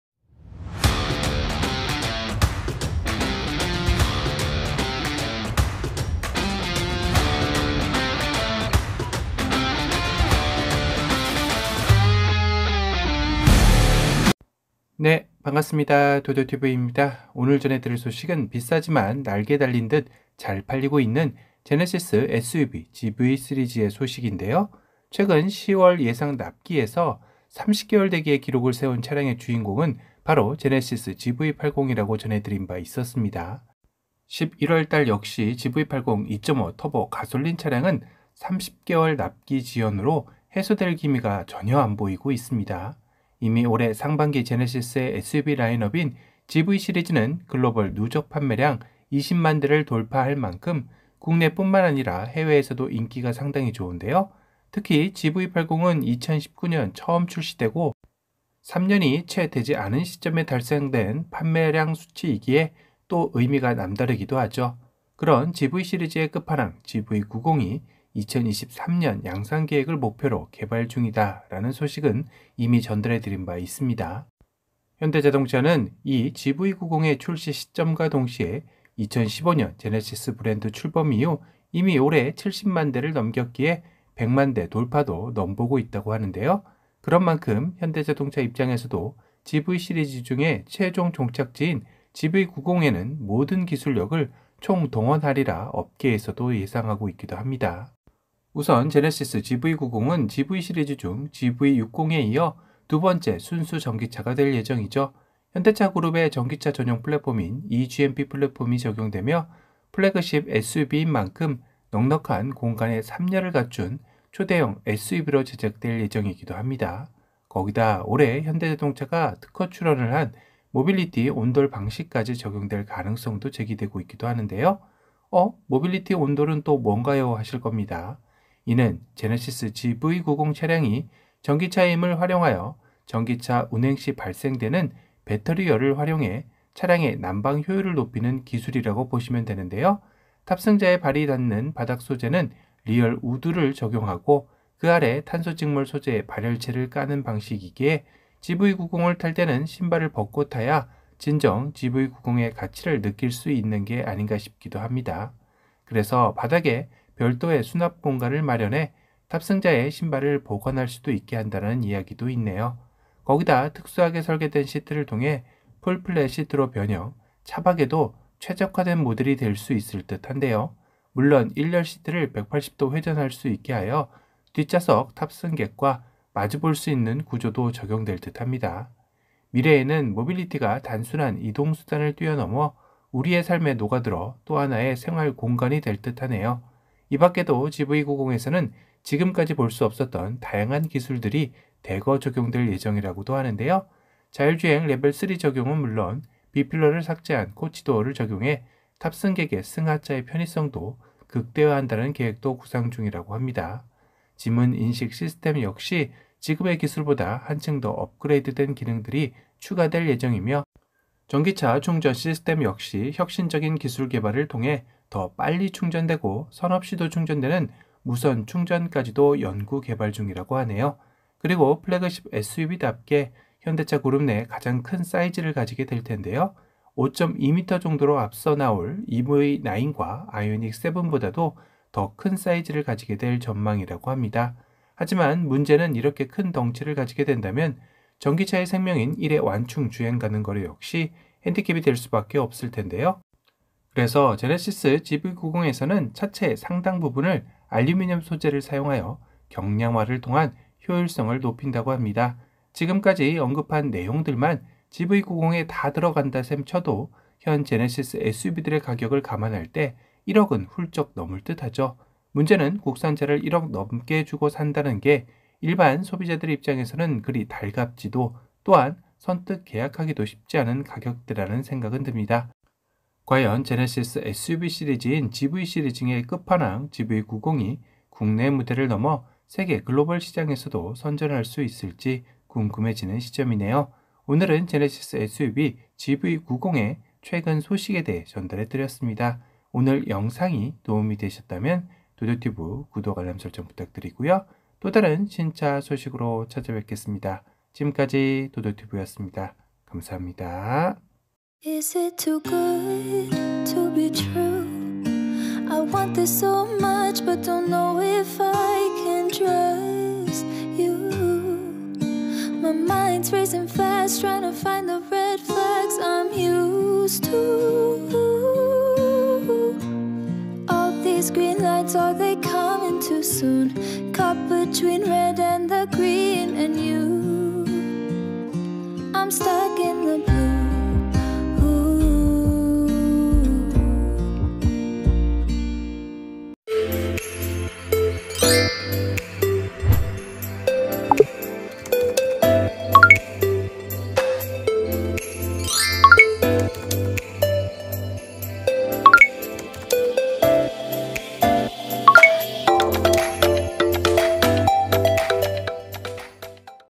제네시스 GV90 이정도는 되야 플래그십? 신발벗고 타는 초호화 럭셔리 SUV!! 모빌리티 온돌 최초적용 최신 신기술 총투입된다~
제네시스 GV90 이정도는 되야 플래그십? 신발벗고 타는 초호화 럭셔리 SUV!! 모빌리티 온돌 최초적용, 무선충전 전기차 등 최신 신기술 총투입된다~